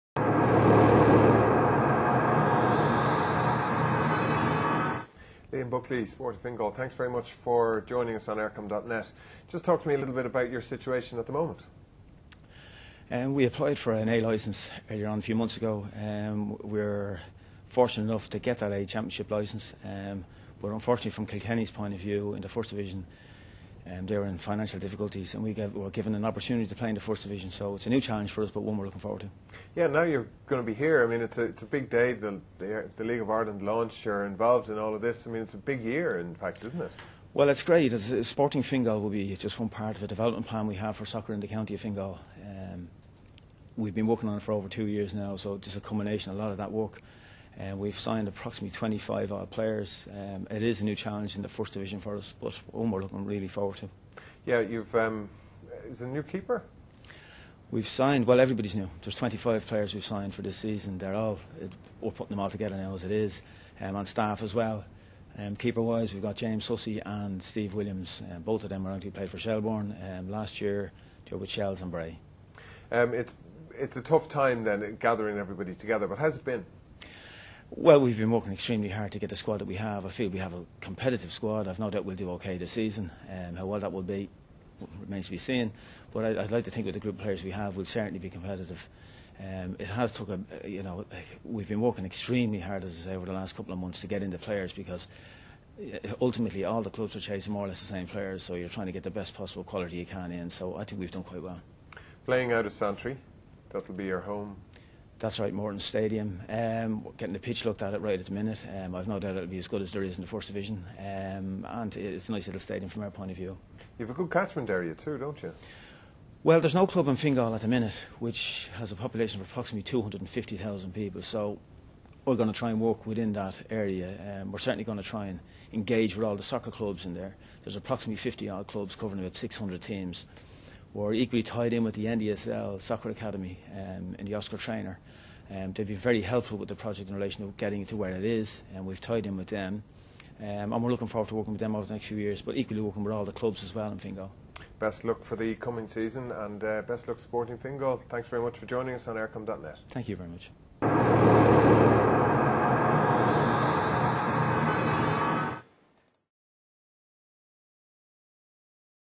Liam Buckley Interview
Laim Buckley talks to eircom.net ahead of the 2008 league of ireland season